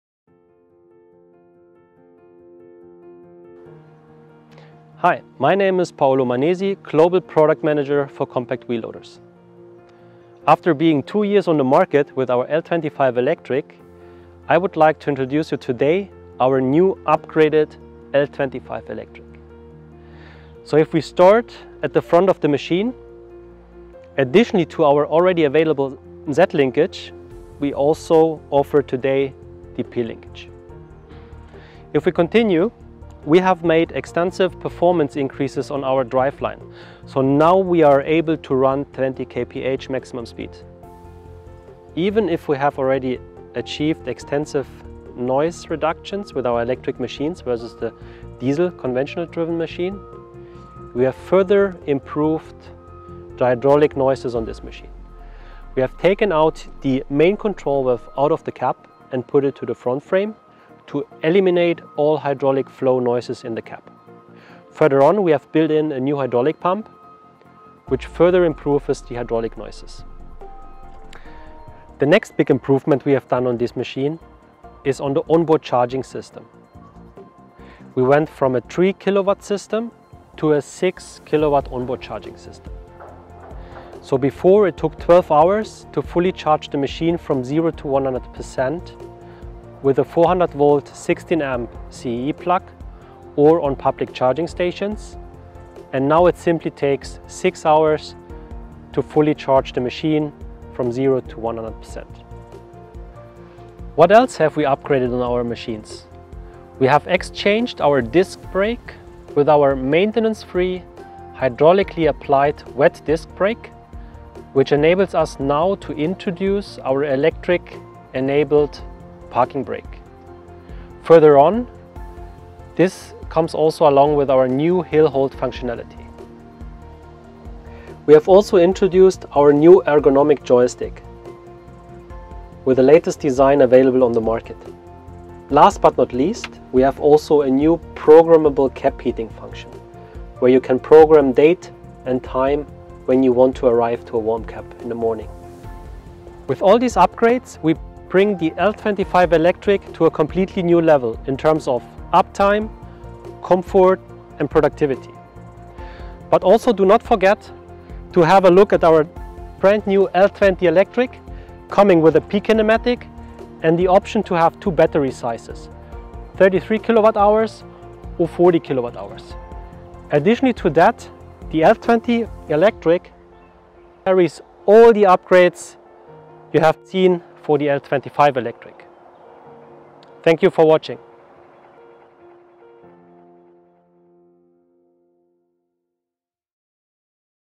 The upgraded Volvo L25 Electric
Watch and discover the key features and benefits of the upgraded Volvo L25 Electric -  The compact electric wheel loader is now being upgraded to ensure even greater uptime and versatility on the job site.

The time it takes to achieve a full charge from zero is now halved to only six hours with the improved 6kW on-board charger – if used with a 400V 16A CEE socket or public charging station.